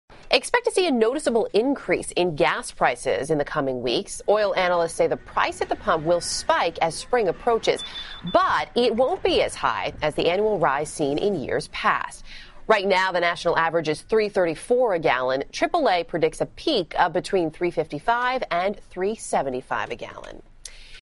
Gas prices set to rise in coming weeks
The current national average for a gallon of gasoline is $3.34, though prices are predicted to rise to as high as $3.75 in some areas.